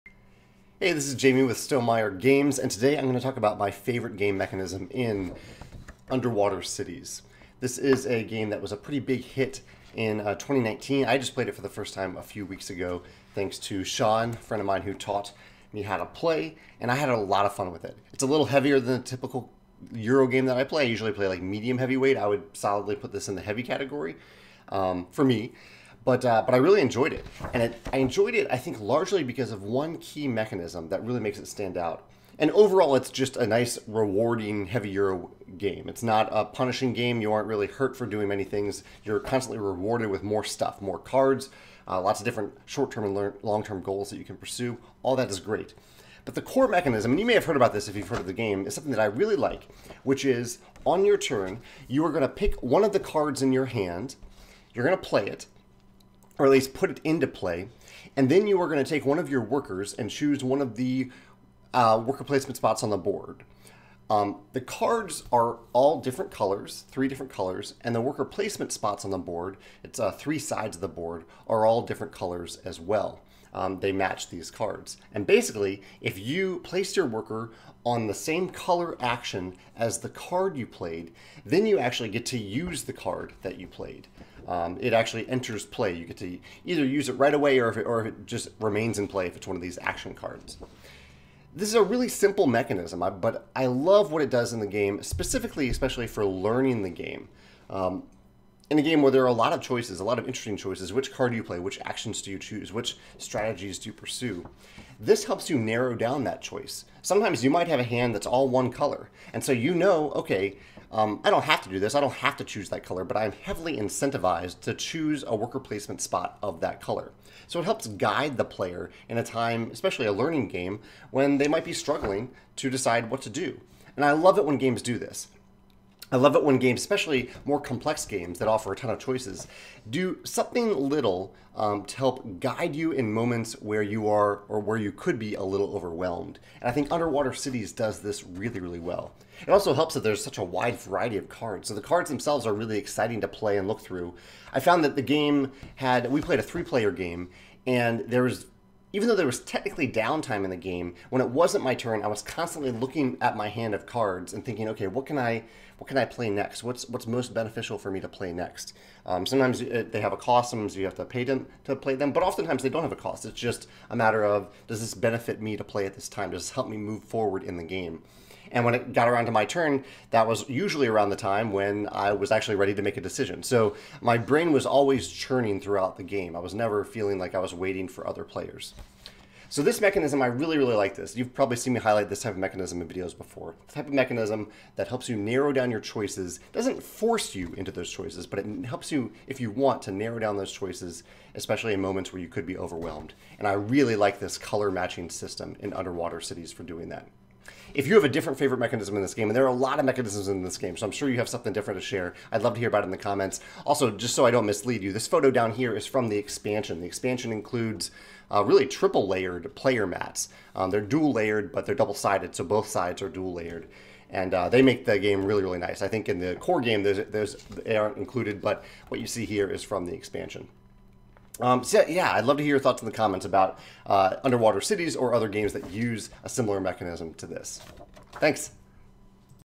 Underwater Cities: My Favorite Game Mechanism
Jamey discusses his favorite mechanism in the excellent heavy Euro game, Underwater Cities.